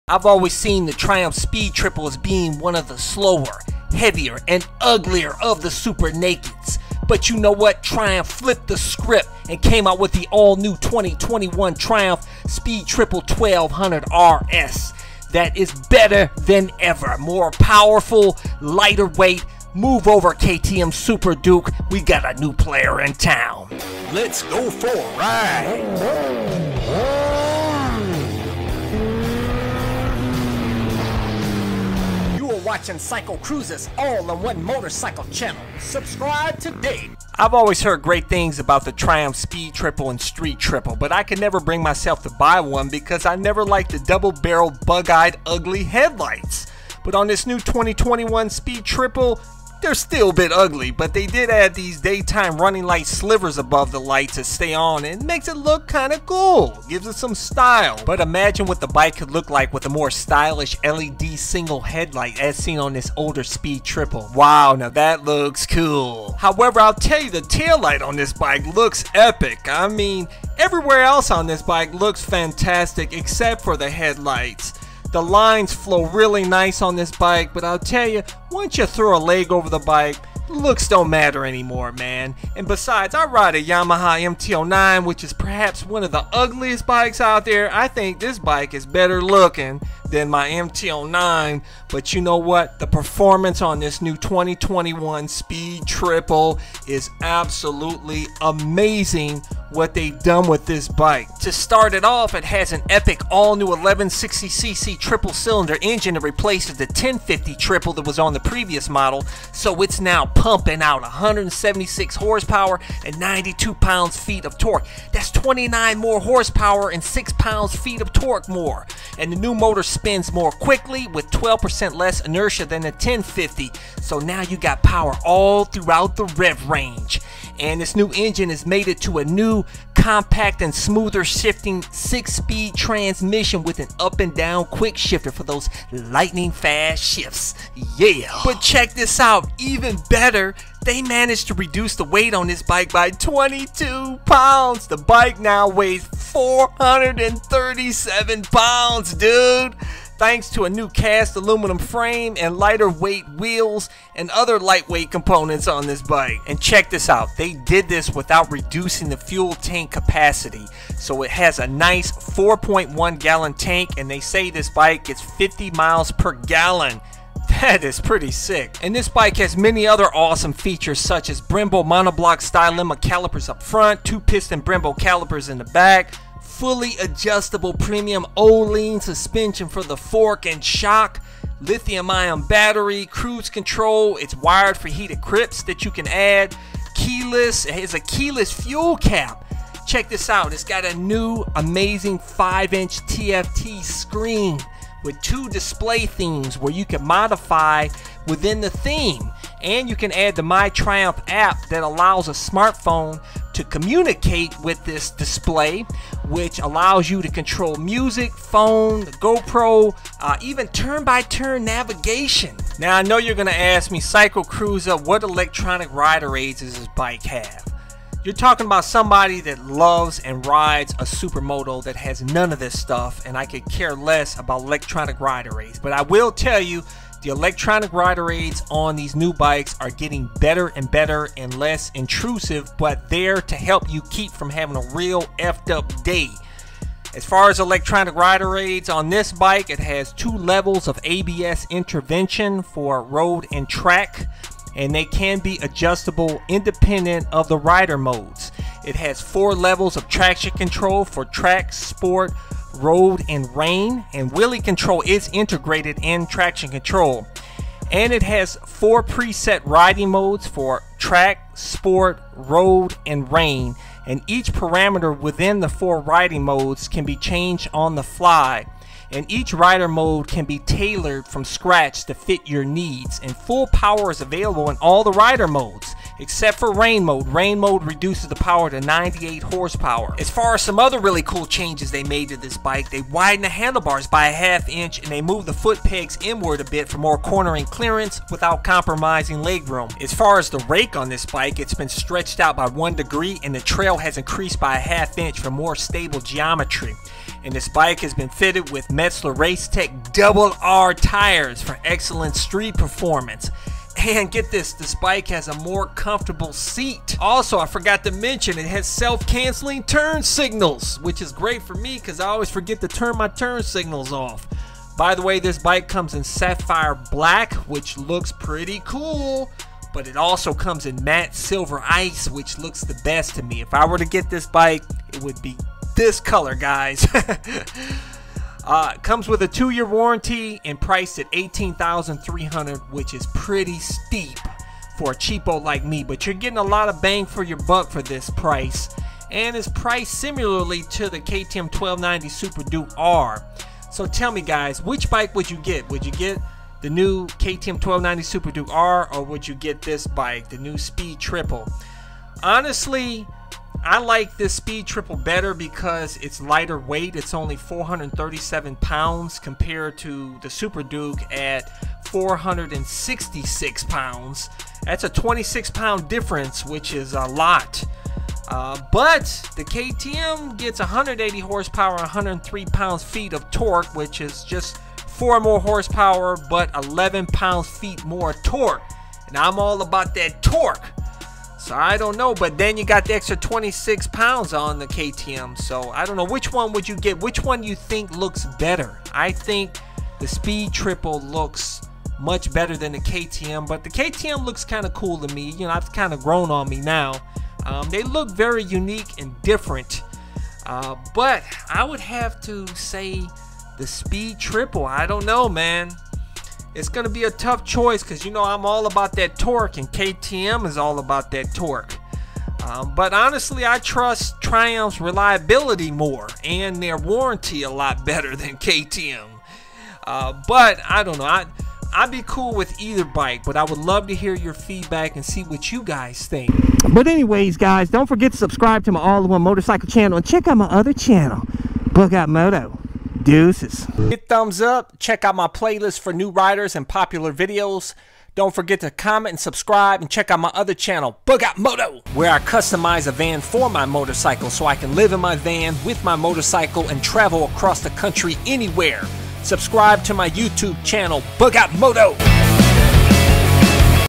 2021 Triumph Speed Triple 1200 RS Review - Better Than Ever!
Review of the all new 2021 Triumph Speed Triple 1200 RS.